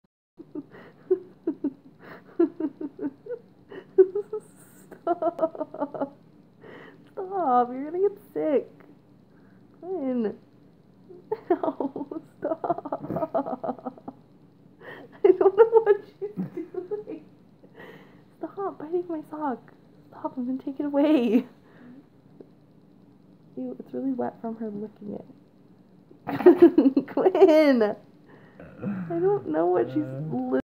Vicious Hedgehog Attacks Human
The human in question was not seriously injured.